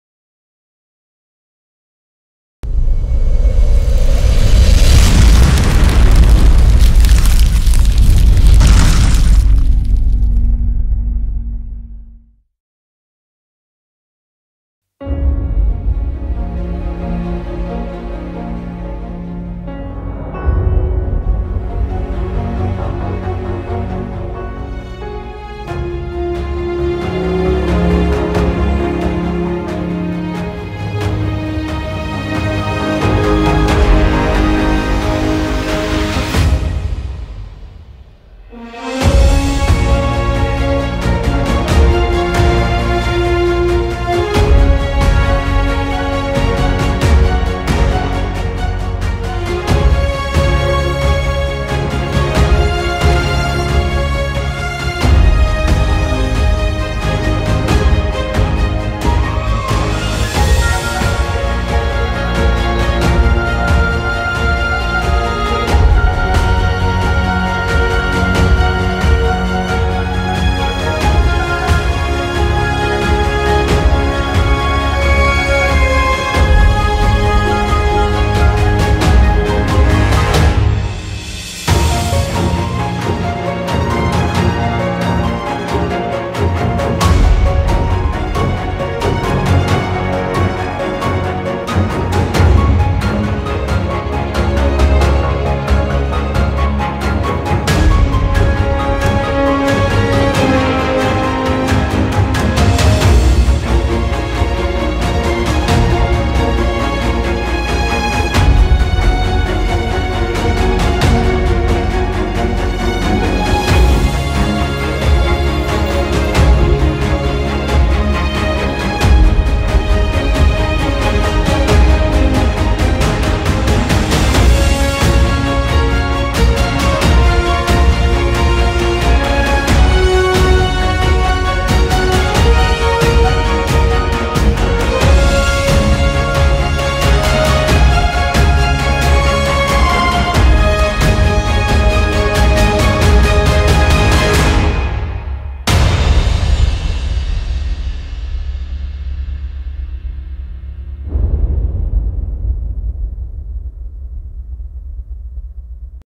Phonamic Aerial Video Reel
- Aerial Video Company - This is the latest video reel from Phonamic, an aerial video and photography company located in Northwest Arkansas.